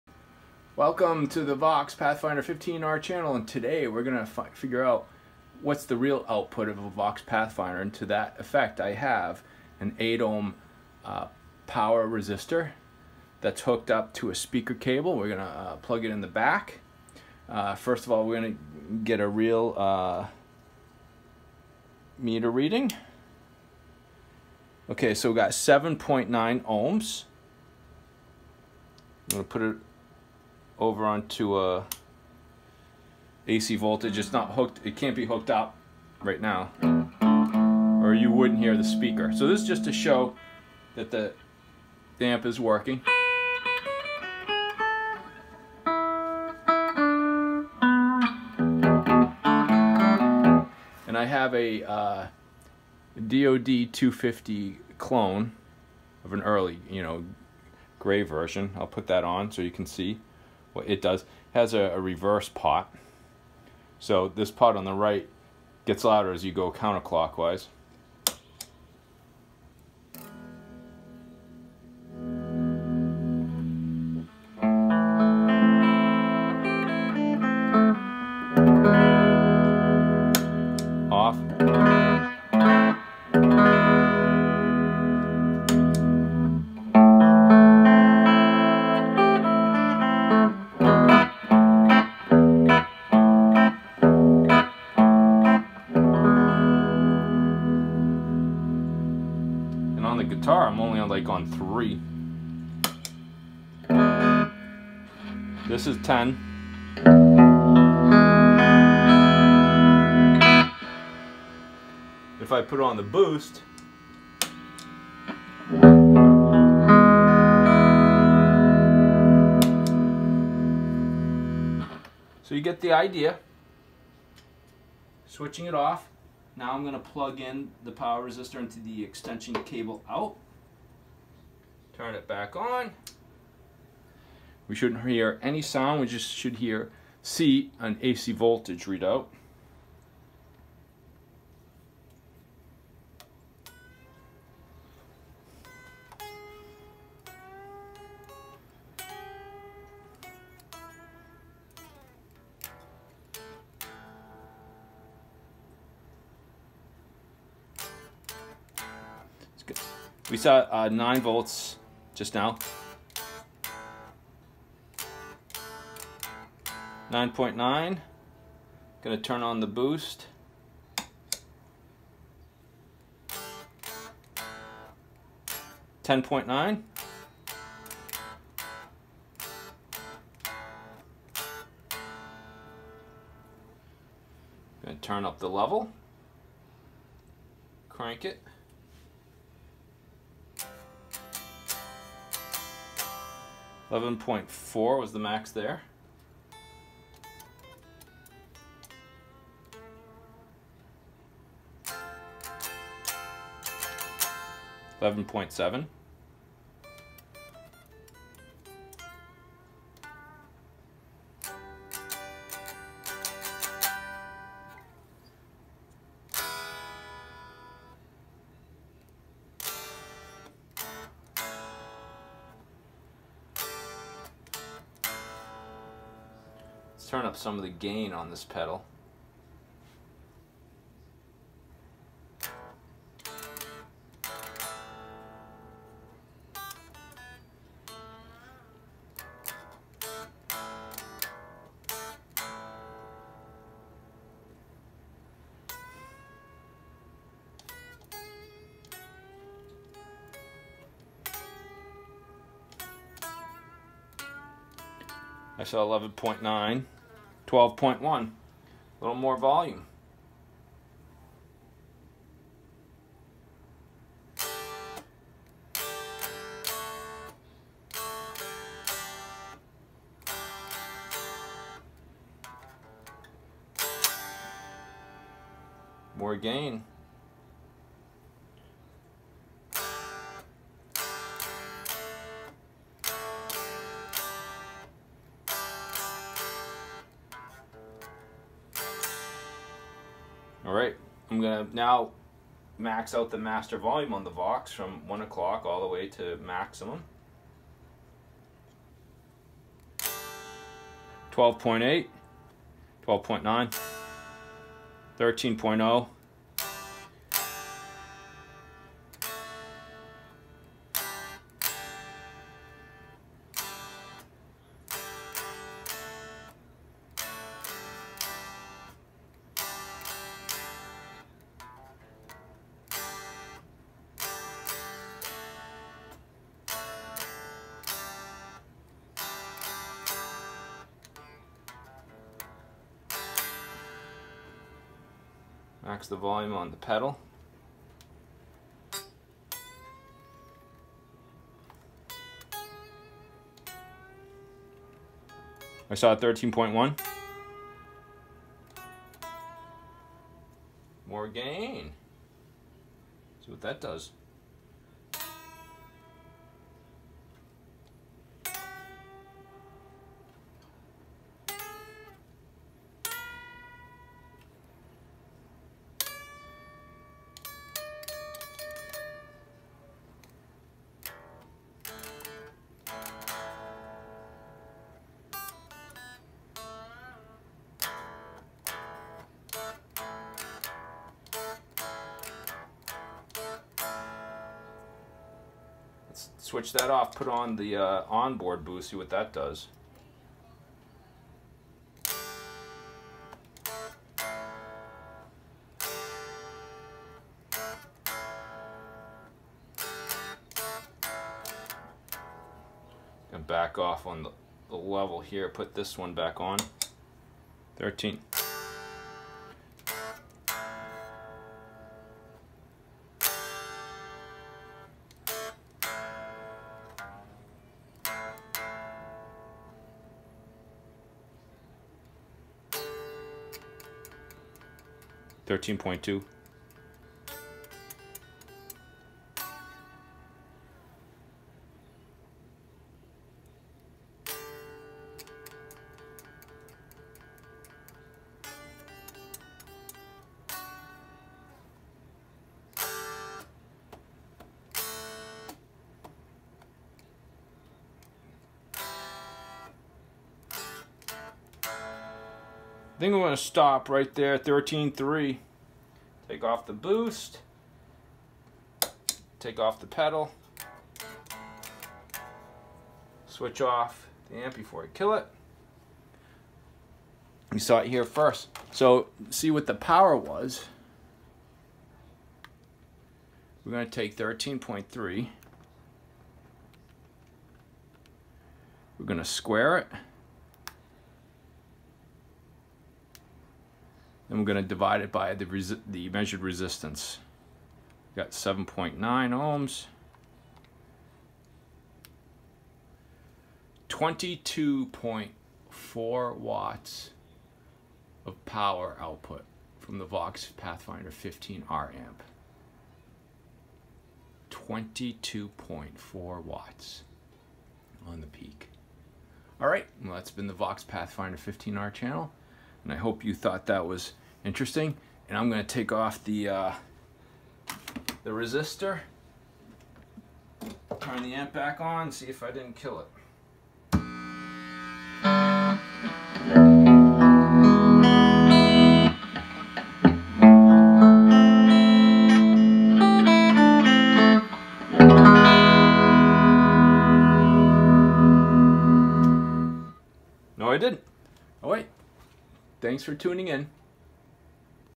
Vox Pathfinder 15R puts out 22.4 watts!!!!
In this video, instead of plugging in a different speaker into the onboard speaker out jack, I use a speaker cable to an 8 ohm power resistor. Without being able to rely on our ears to see if the amp is outputting any power, we’re relying on the magic of a multimeter to show us the incredible amount of power from this little practice amp!  Don’t poo poo this amp, it’s a little monster!!